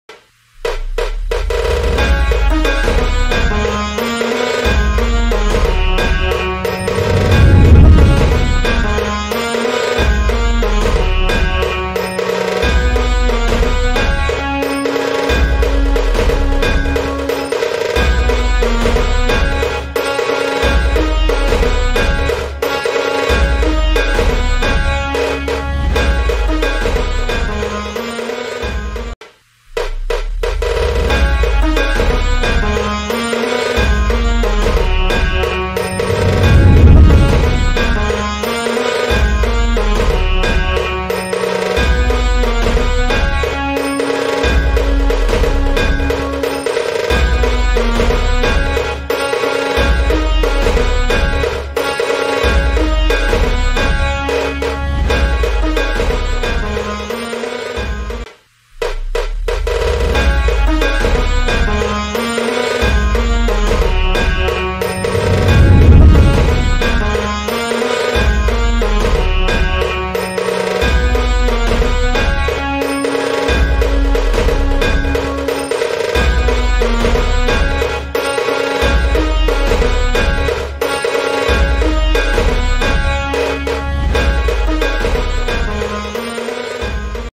Background music used in vinayak mali videos
This video is uploaded only for the new creators or youtubers who use the music in there videos.
The only purpose of thi video is here.